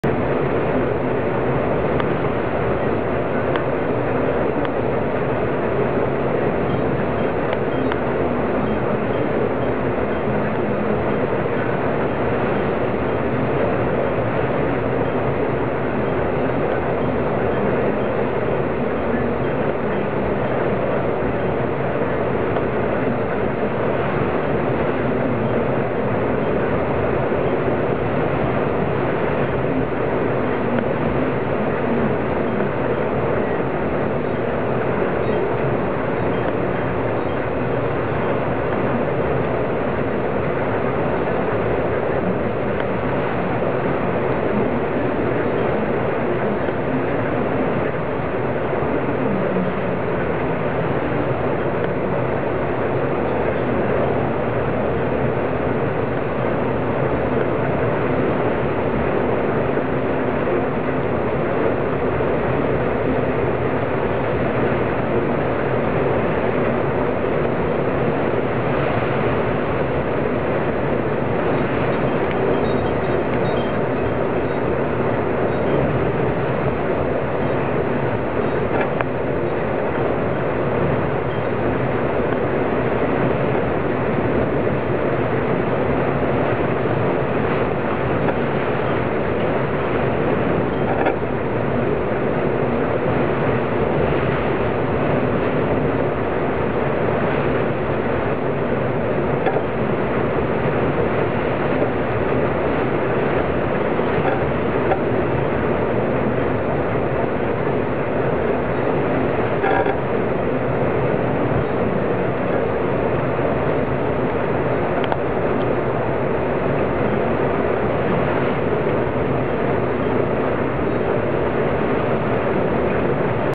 Longships Lighthouse, morning storms , 10th March 2008
Rough sea and 70mph winds hitting the Longships Lighthouse